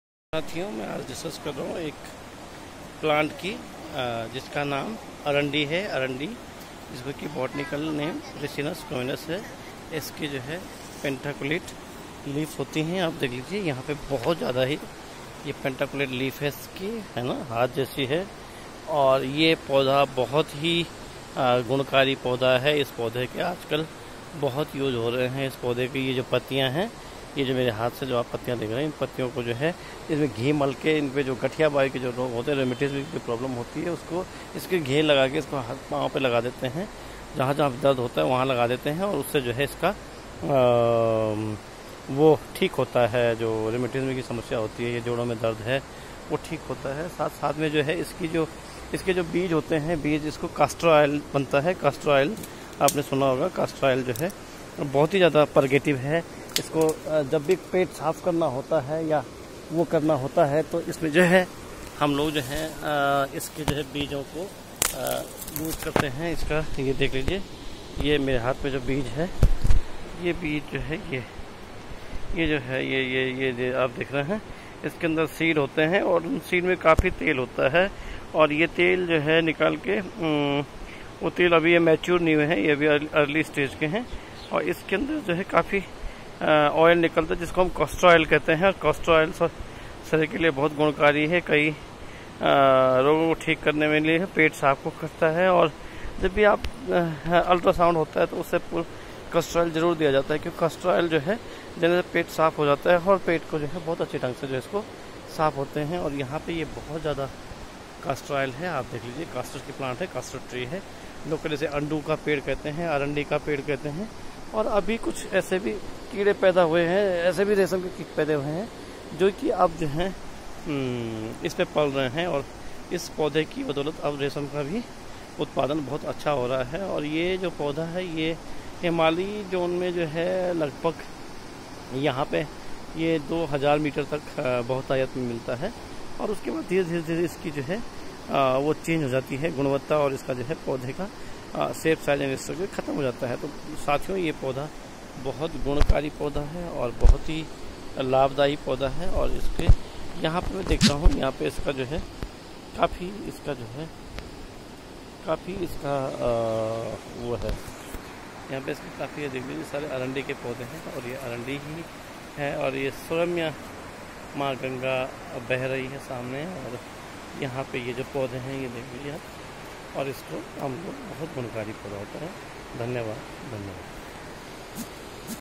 Ricinus communis
अरंडी, अनडू

The plant is known to display antimicrobial activity and has been used to treat several ailments [66]. Its leaf, root, and seed oil are used in inflammation treatment, liver disorders, hypoglycemic, and as a laxative [68,69] [66]. In Tunisia, the plant is used as a contraceptive [66].